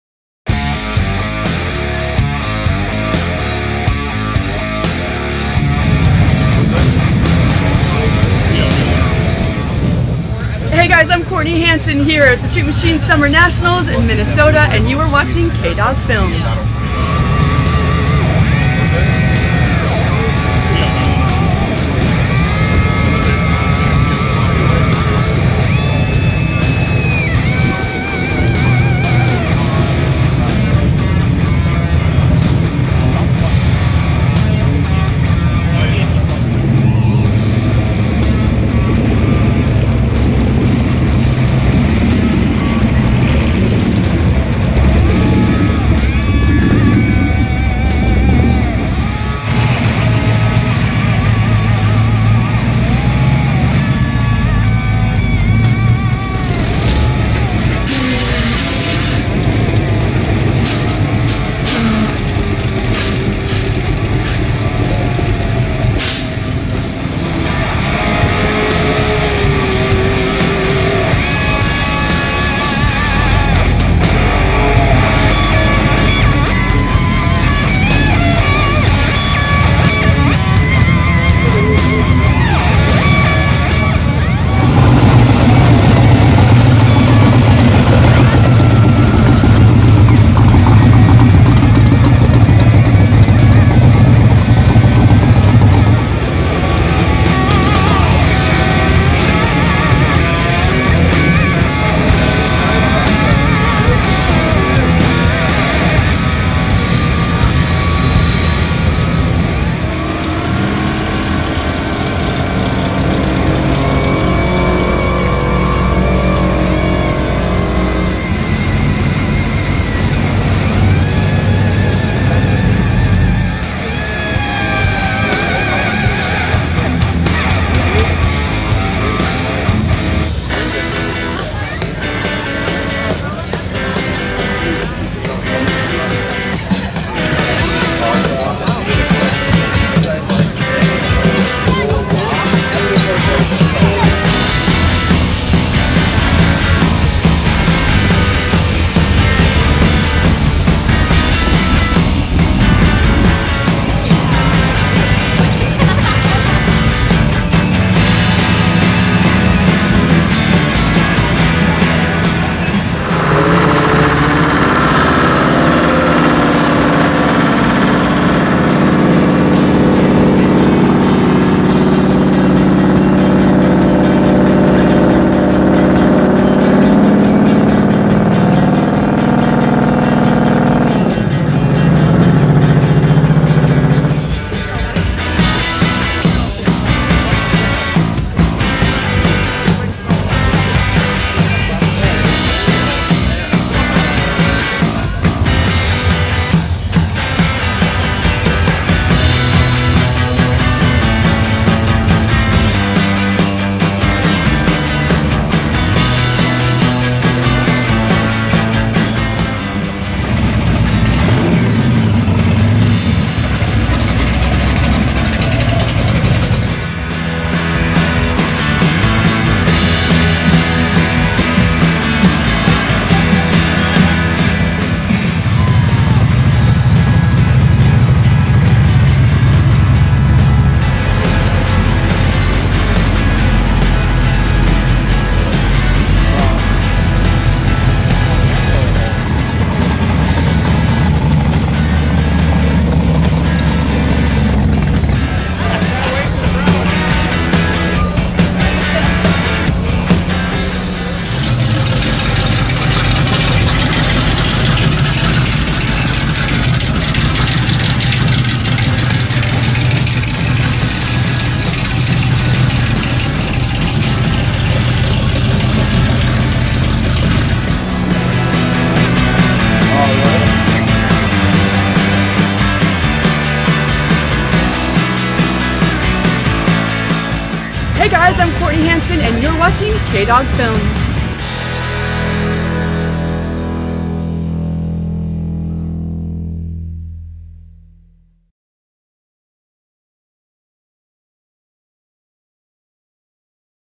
Street Machine Summer Nationals
Cruising the streets of the Minnesota State Fairgrounds at the O’Reilly Street Machine Summer Nationals. See some of the 7,000 street machines that attended this year’s event. It’s the horsepower party of the year.